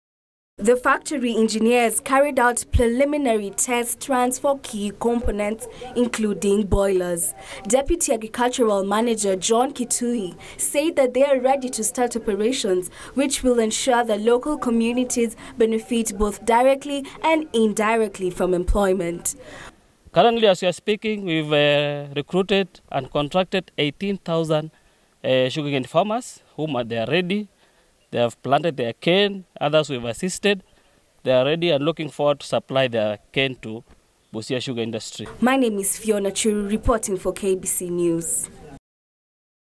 Busia County to start sugar milling
Busia sugar factory in Busibwabo ward, Matayos Constituency, Busia County is set to start milling anytime from now after being  granted an operating licence by the Ministry of Agriculture. The permit was issued just a week after Deputy President William Ruto inspected the facility and ordered Agriculture CS Mwangi Kiunjuri to ensure the miller starts operations not later that this month.